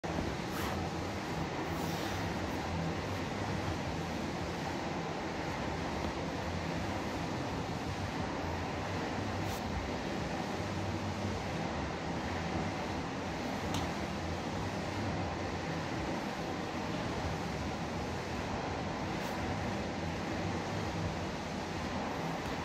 Armbar Supine to Prone
Press KB up into armbar position while laying on your back, slowly roll over to your stomach, as your shoulder blade wraps around your ribcage